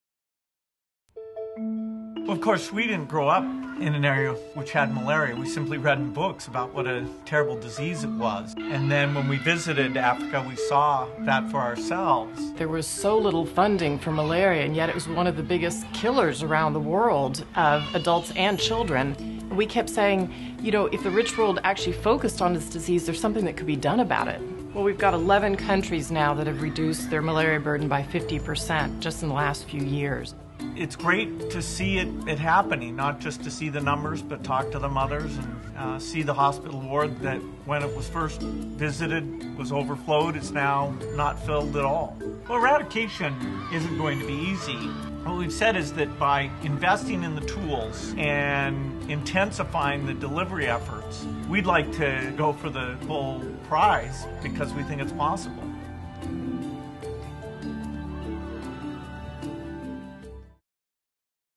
Bill and Melinda Gates on Fighting Malaria
Bill and Melinda Gates discuss why they fight malaria. Bagamoyo, Tanzania, 2011.